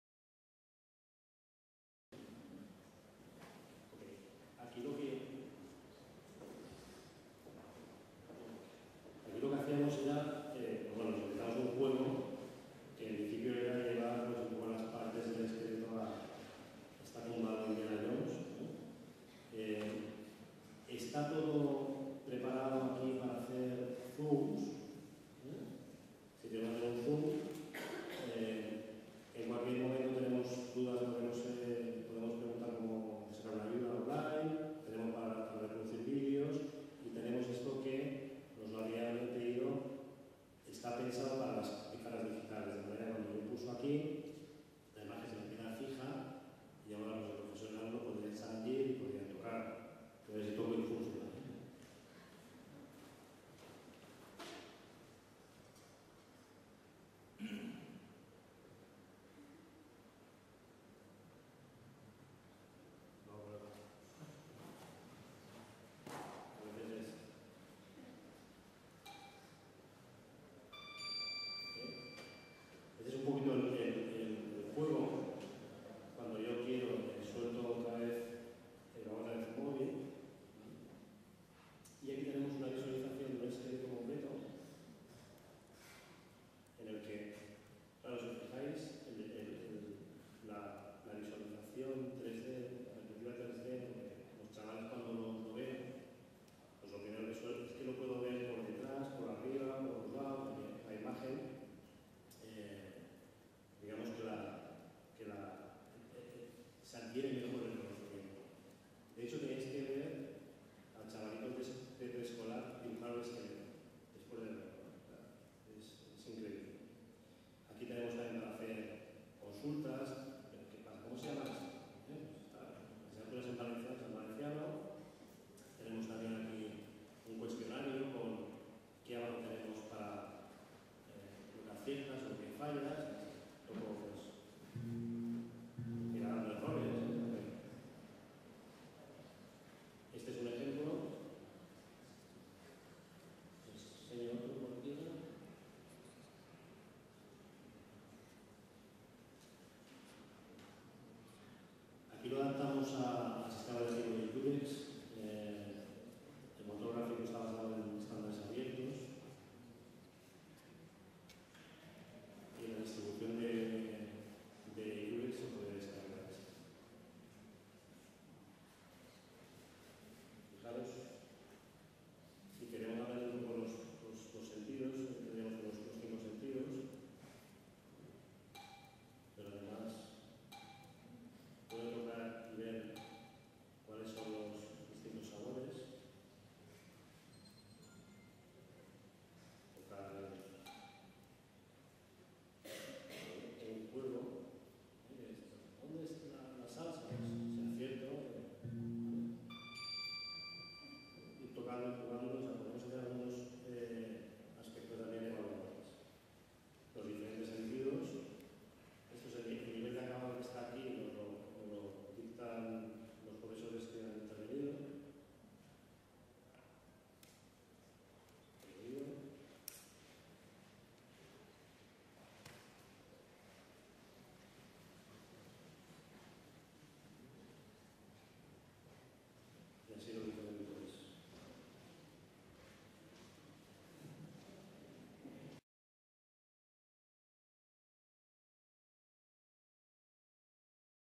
V Jornadas CAMPUSPDI. Bienetec. Realidad aumentada 2/3
Segunda parte de la ponencia de Esteban Anguita  en las V Jornadas CAMPUSPDI Valencia 2012:

"Realidad aumentada en el entorno educativo"